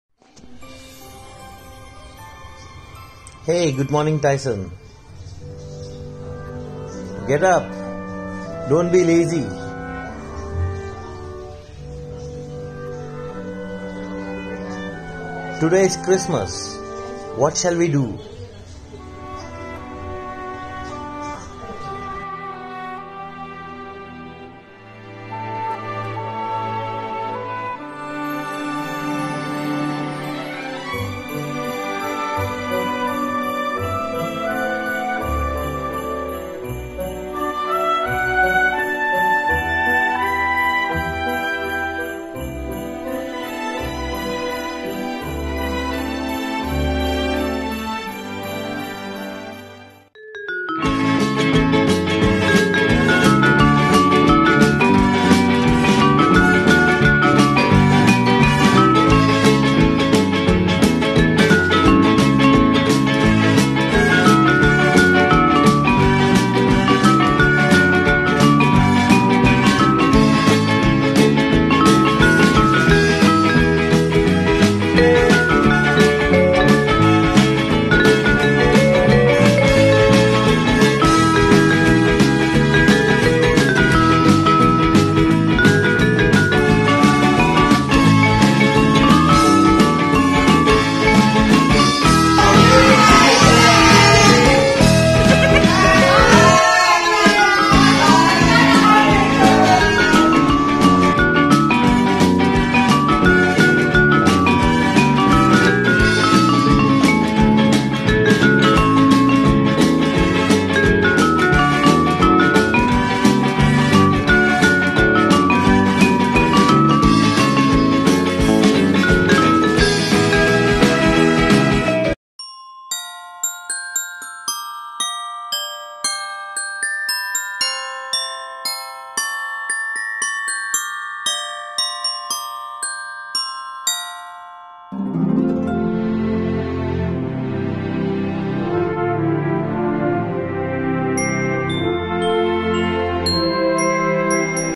Tyson's christmas | Labrador plays Santa for this Christmas
Tyson is a loving Labrador, celebrates Christmas with his best buddies. He loves to play with children in our neighborhood and the children come to him whenever they are low and Tyson cheers them up. Hope he continues to bring many more smiles.